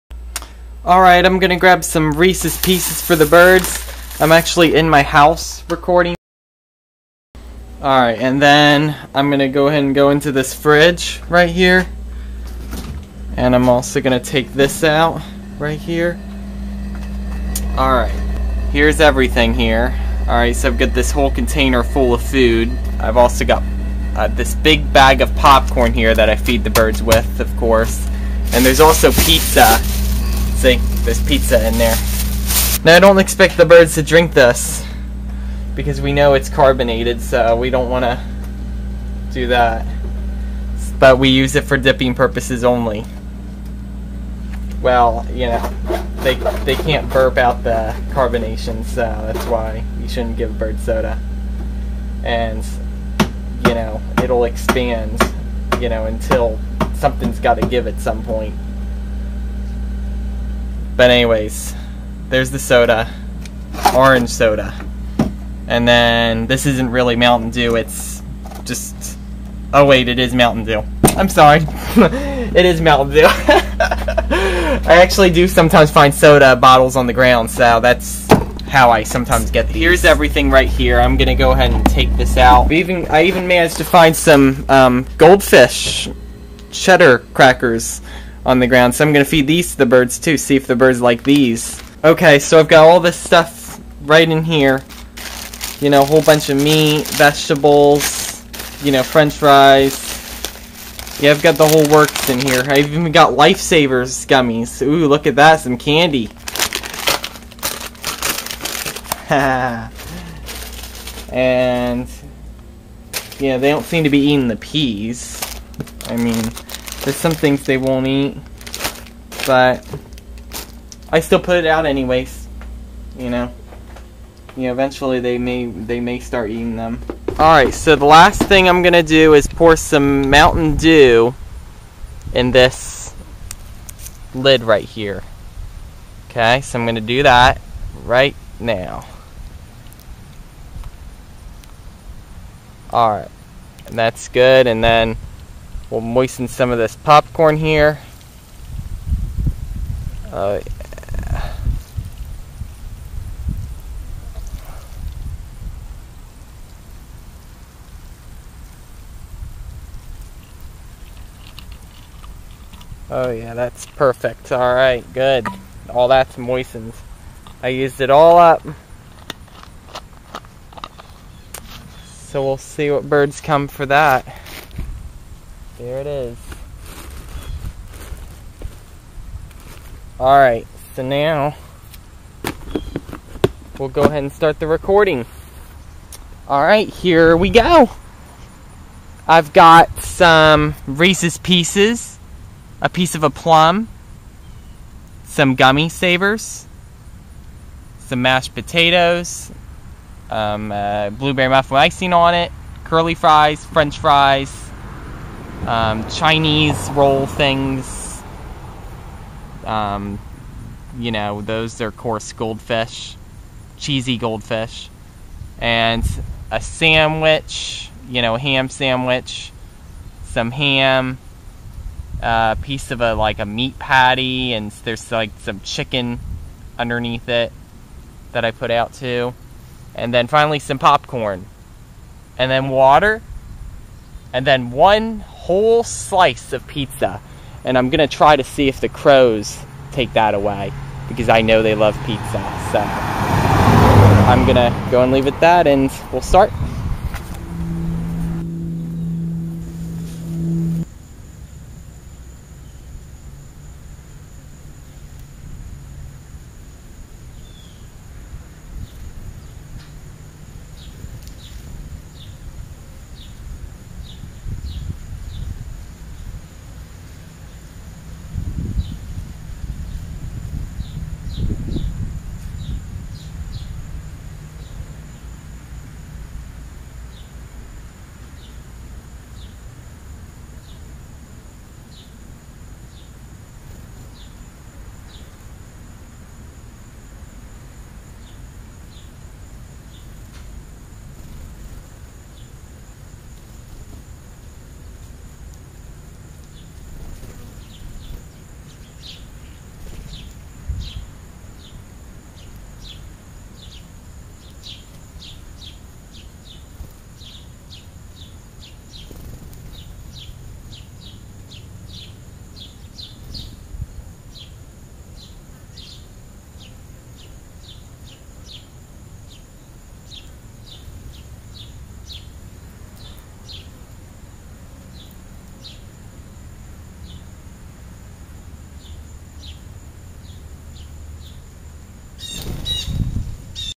Bird Feeding August 16, 2012
There was like, no birds around, until Part 3, so I decided to use Windows Live Movie Maker, instead of the DVDVideoSoft video trimmer that I normally use. It's strange that there weren't any birds around. Yesterday, there was so many of them around. I don't know what's going on. I blame the stormy weather again. Well, all that food, especially the meat, is going to go bad because I put more out than yesterday. I put it all back in the fridge as soon as the camera stopped. Keep in mind, I had the meat in the fridge for 2 weeks now. There was a crow that started crossing the street a few hours afterwards, then when a car came by, it flew away. I wonder if it would have ate some of the food if I had left it there? It never came to the scraps of popcorn that were left so that probably answers my question.

This is really short. I decided to do a special intro this time showing the food in my fridge. I feel that since I am getting actual viewers watching my videos, I feel that I should take more time to explaining things.